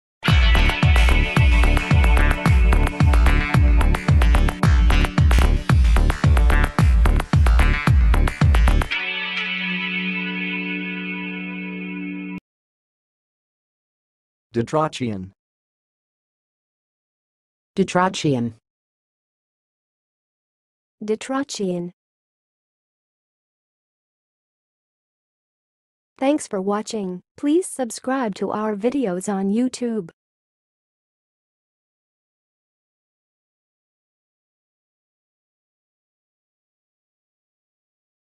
How to say "ditrochean"! (High Quality Voices)
Watch in this video how to say and pronounce "ditrochean"!